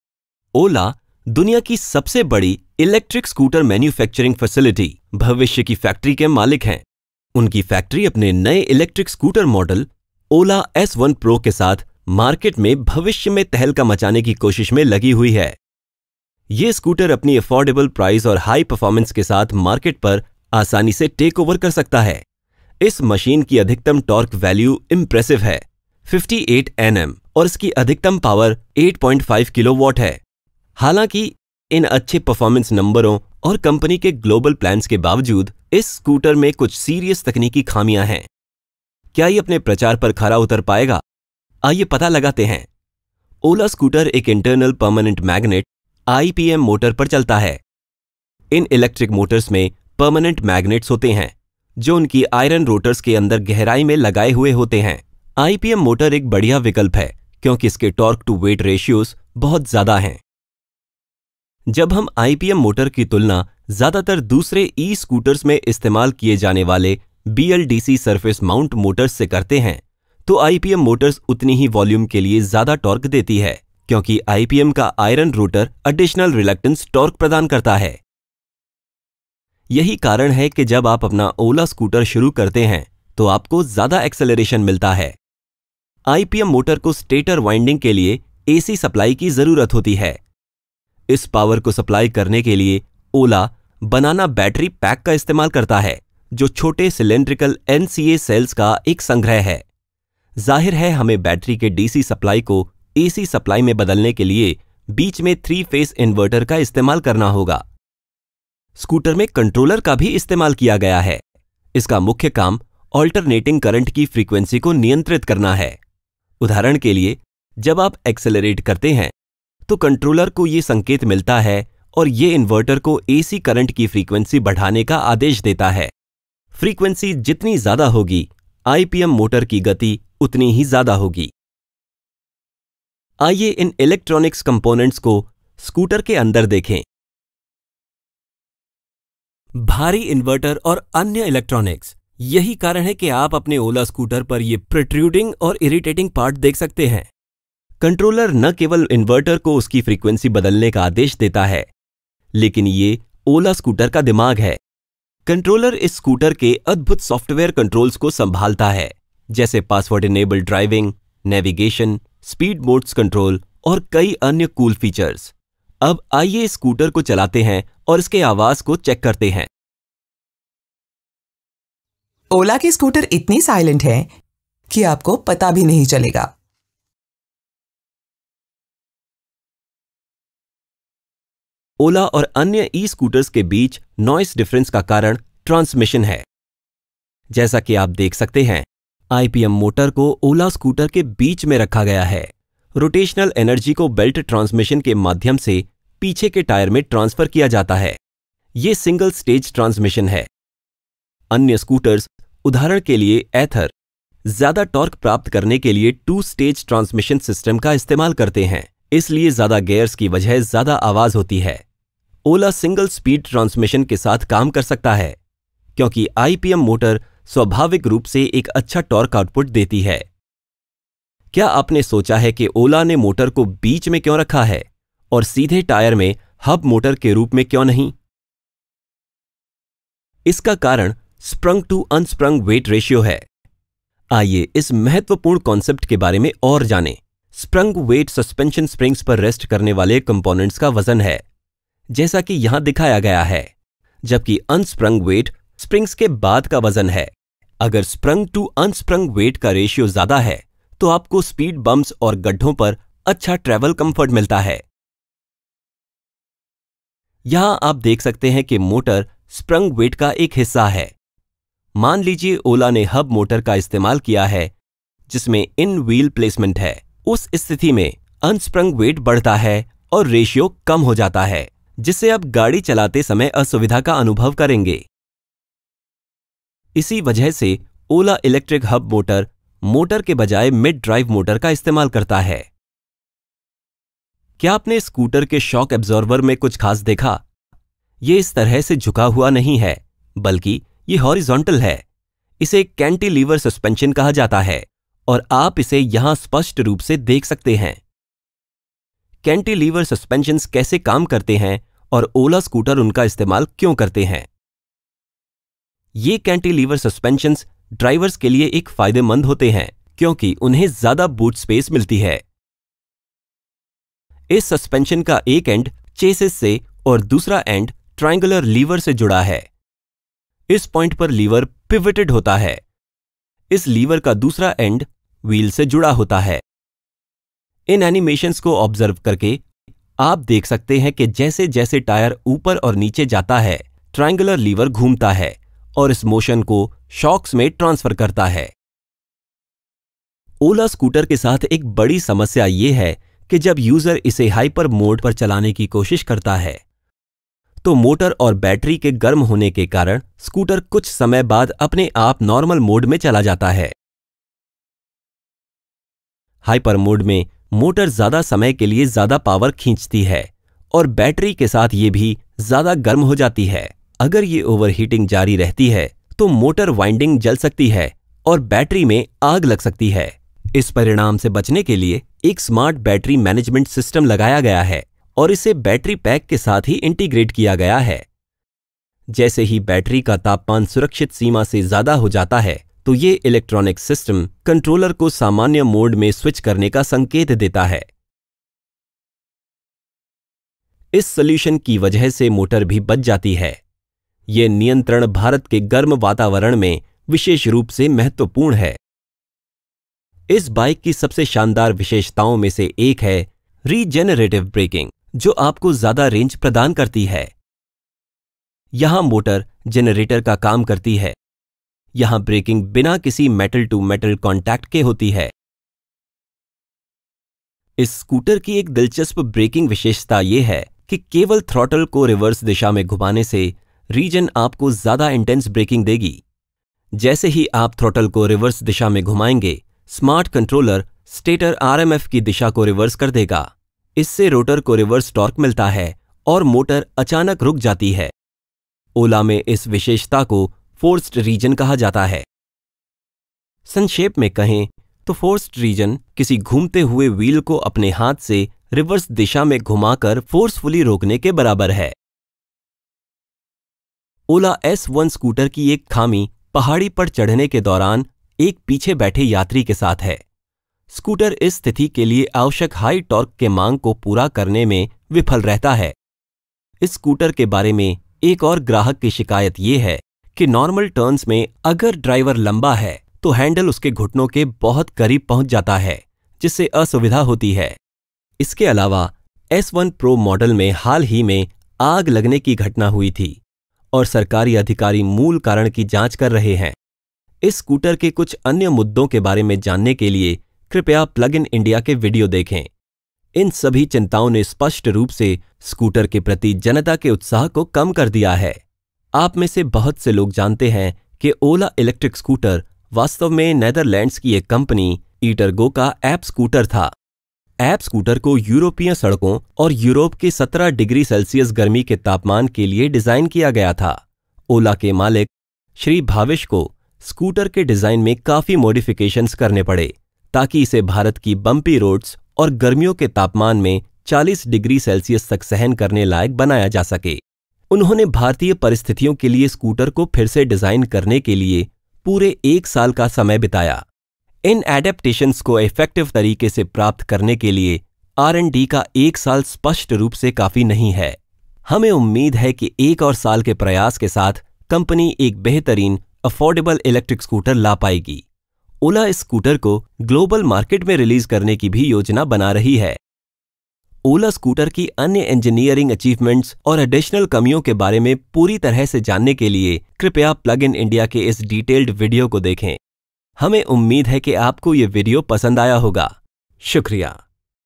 Ola scooter | इसके पीछे की detailed engineering
Ola के पास दुनिया की सबसे बड़ी electric scooter manufacturing facility है। क्या वे मार्केट में हंगामा कर पाएंगे? आइए पता लगाते हैं।
Ola scooter के बारे में ज्यादा जानने के लिए PluginIndia के इस वीडियो को देखें